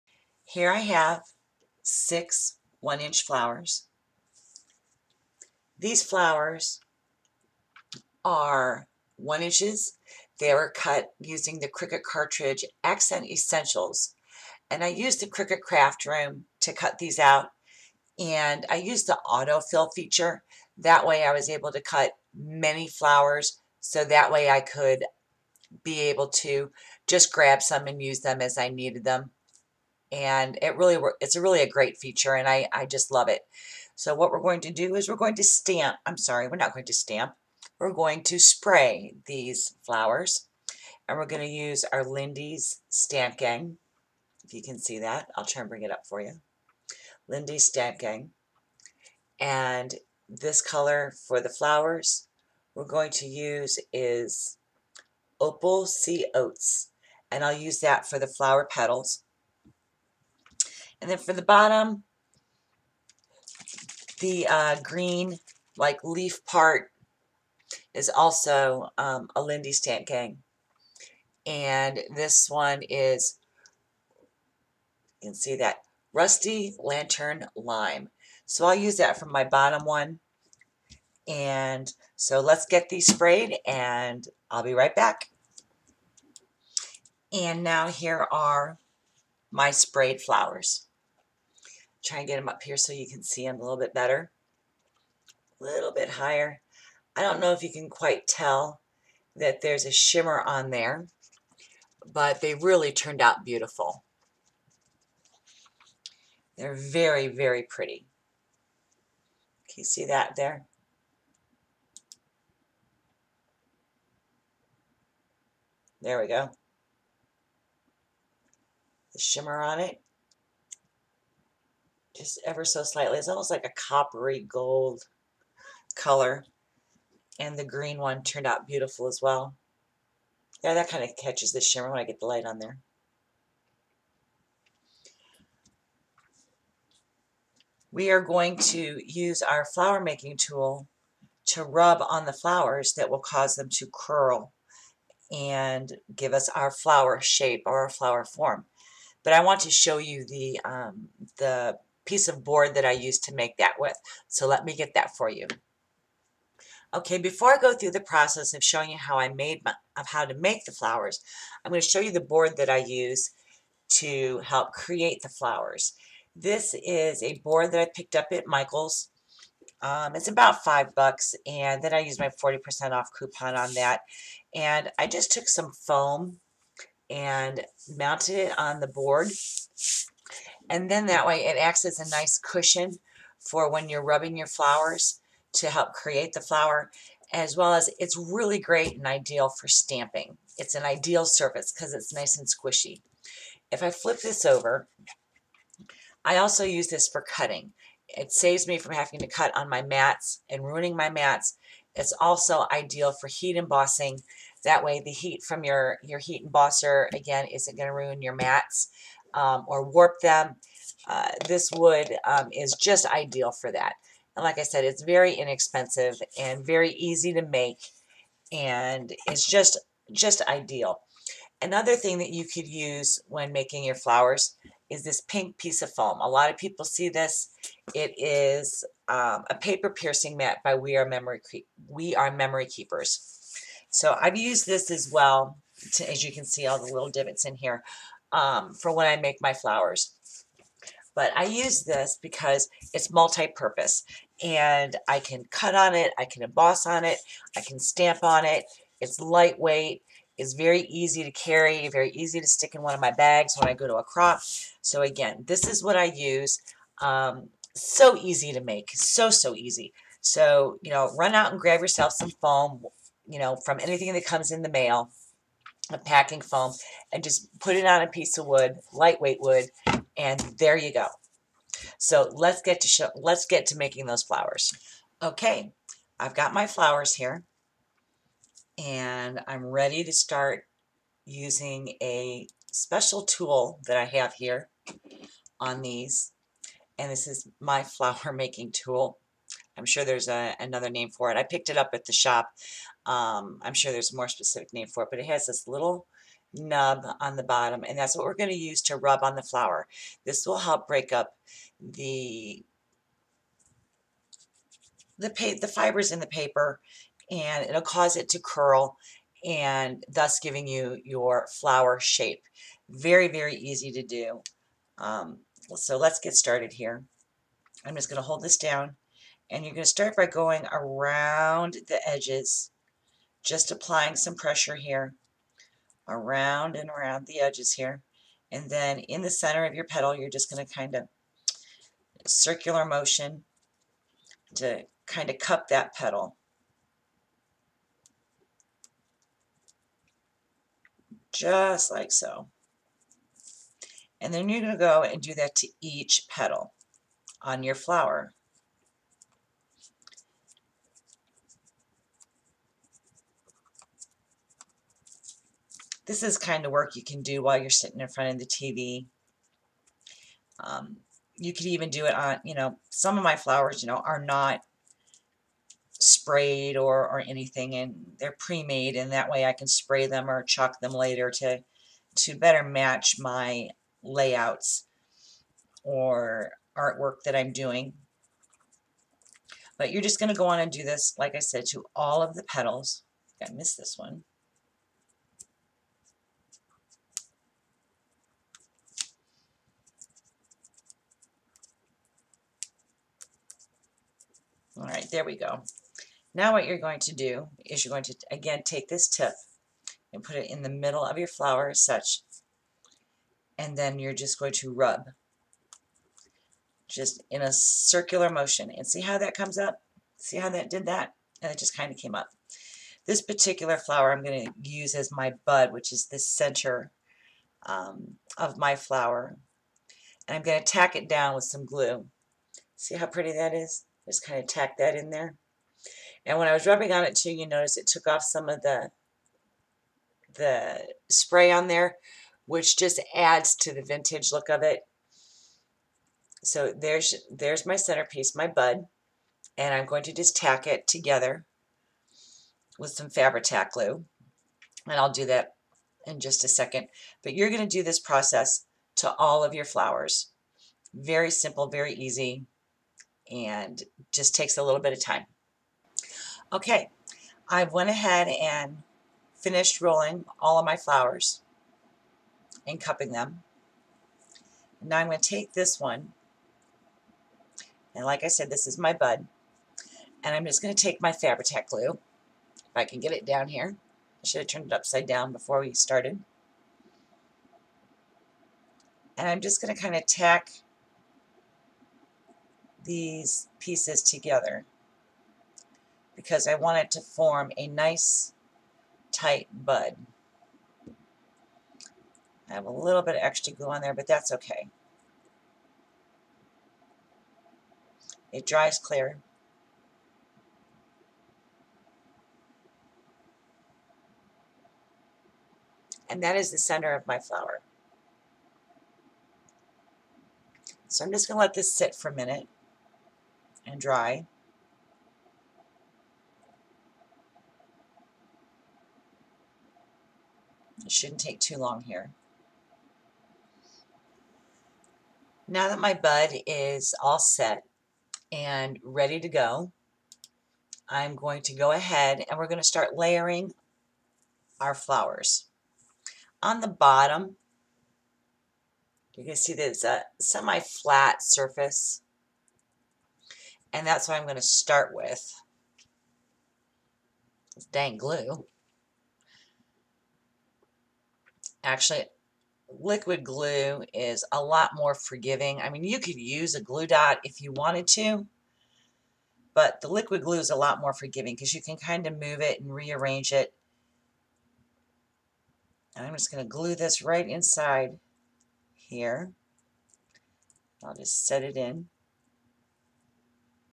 GoldenHands: self-made vintage amazing flower v.1
Today I will show you how to make a vintage amazing flower at home without any factories or mashines